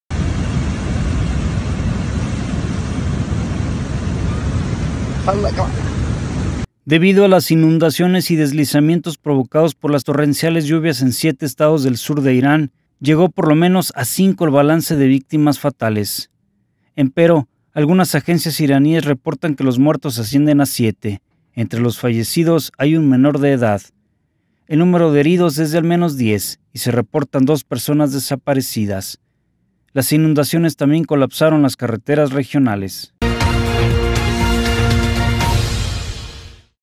Irán: muertes por inundaciones
Lluvias torrenciales, inundaciones y deslizamientos afectan al sur de Irán; por lo menos hay cinco personas sin vida y dos desaparecidas.